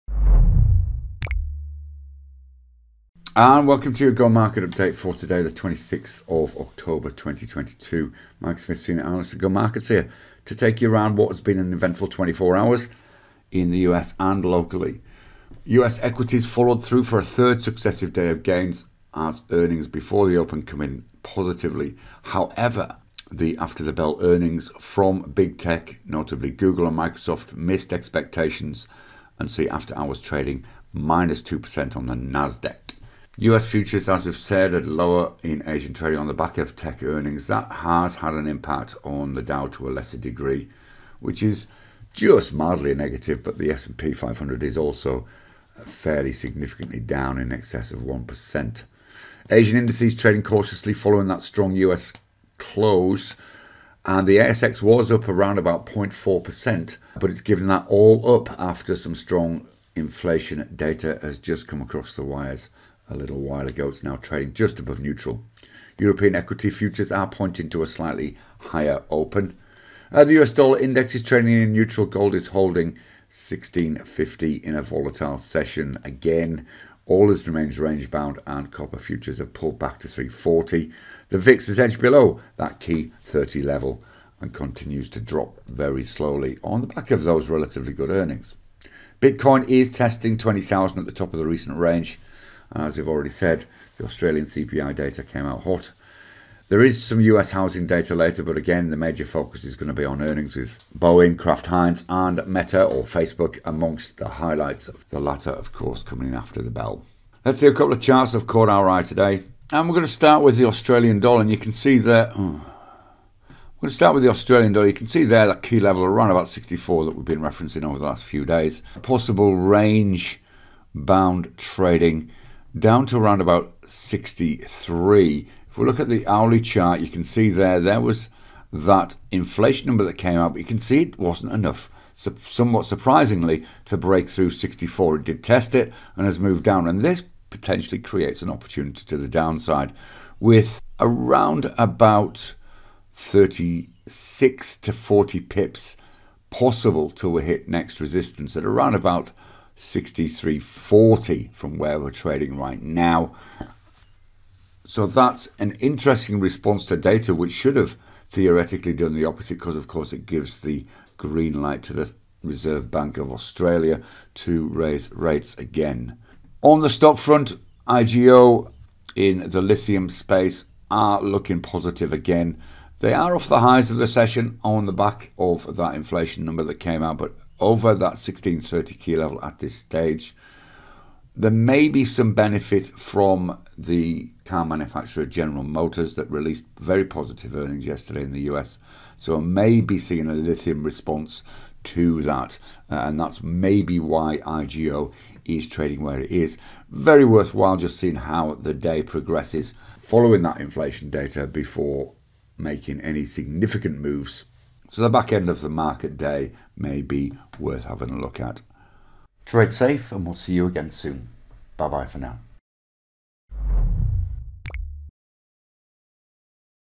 GO Update and Charts of Interest - AUDUSD, IGO.asx
With US earnings dominating sentiment and a 32yr high in Aussie CPI we look at marekts today with Senior Analyst Mike Smith